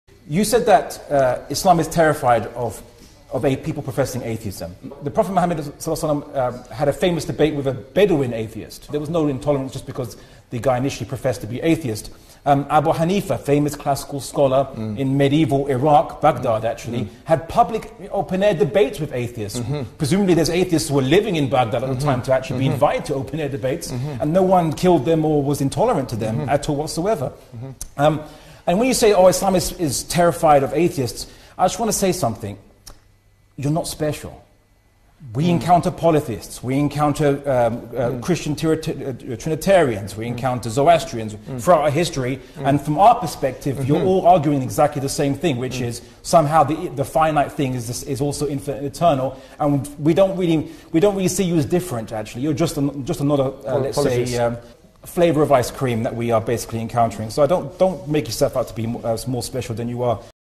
Are Atheists special ? - Abdullah Al Andalusi
A highlight from Abdullah AL Andalusi in a debate in Oxford.